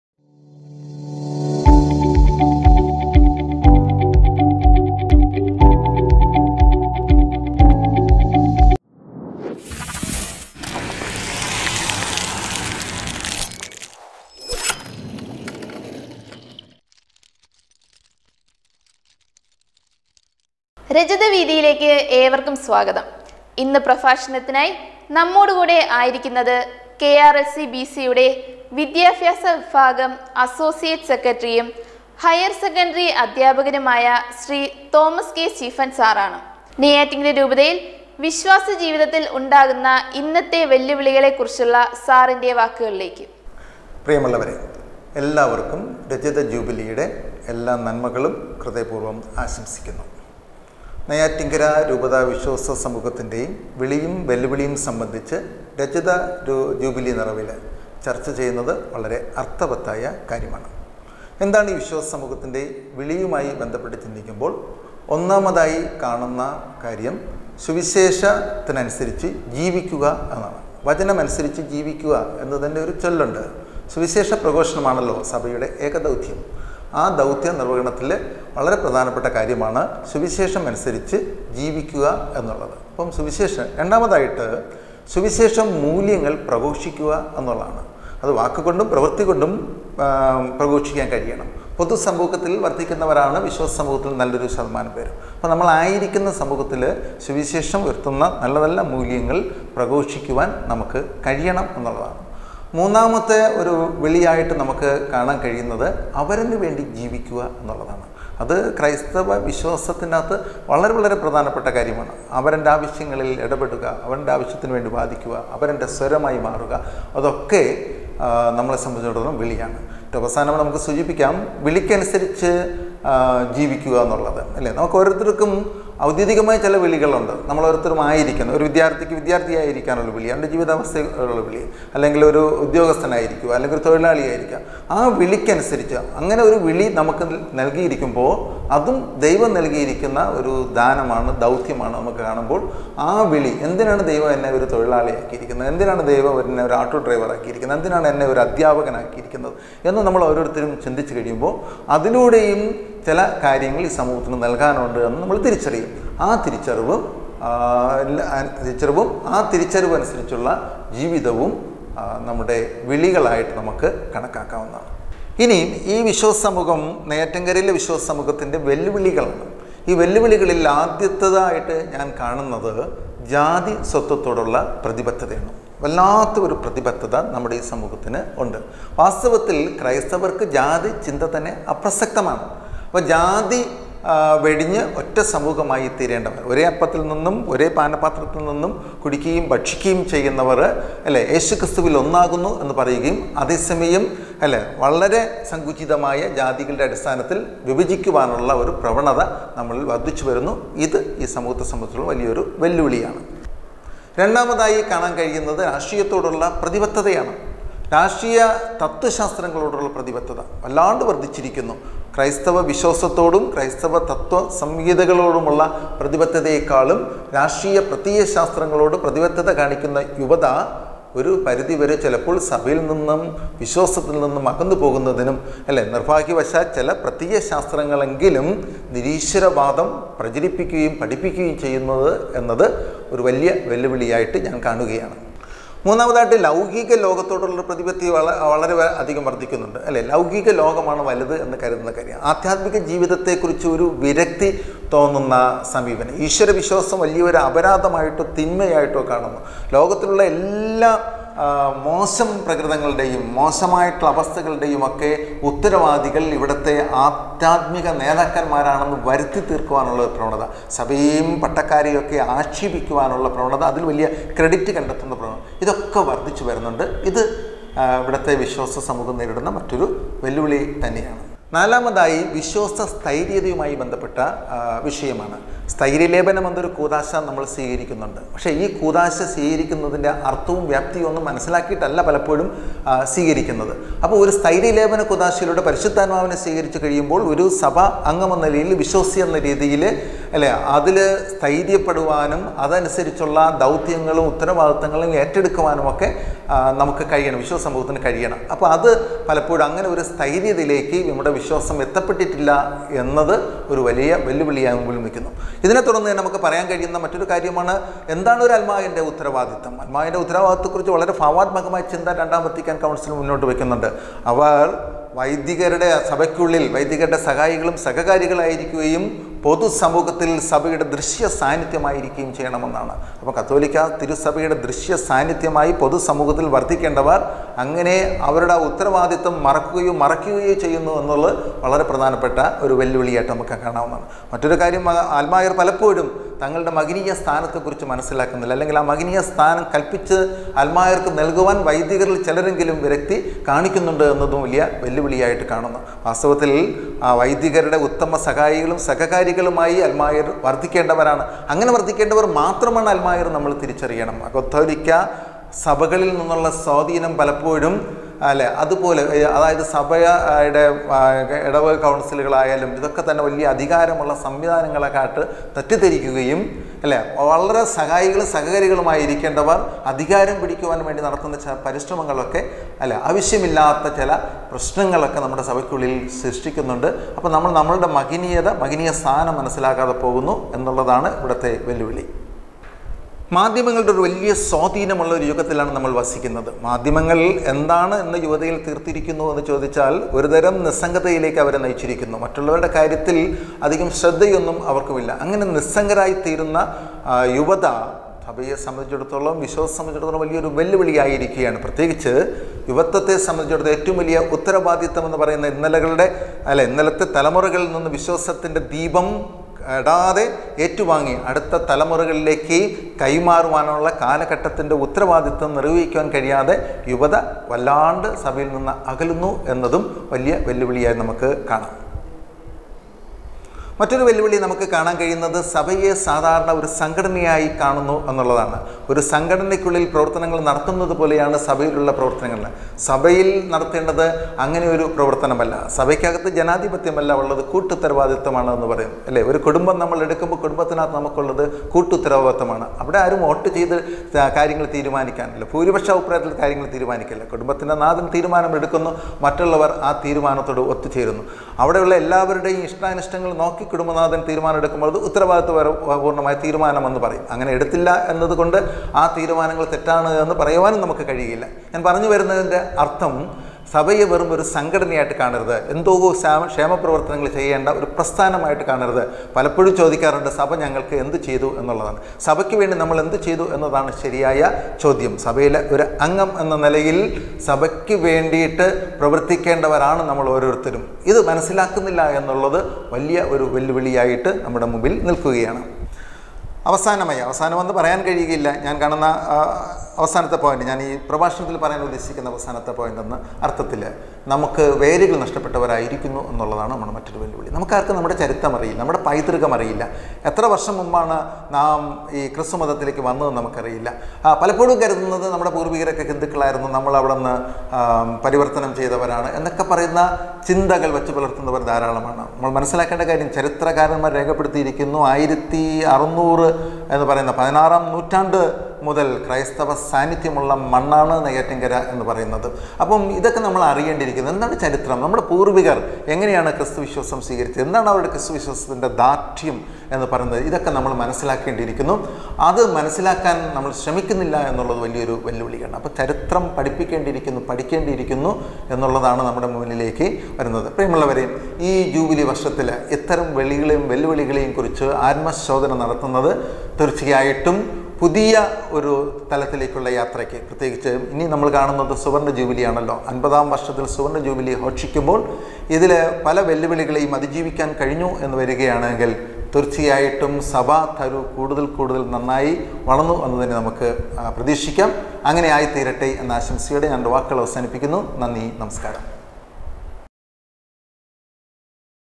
രജതവീഥി - 5 II Mr. Thomas K. Stephen II വിശ്വാസ ജീവിതത്തിൽ ഉണ്ടാകുന്ന.... II 10.10.2021
രജതവീഥി - 5 II Mr. Thomas K. Stephen II  വിശ്വാസ ജീവിതത്തിൽ ഉണ്ടാകുന്ന ഇന്നത്തെ വെല്ലുവിളികൾ II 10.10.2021 II II Sanjo Communications  Official YouTube media platform of the Latin diocese of Neyyattinkara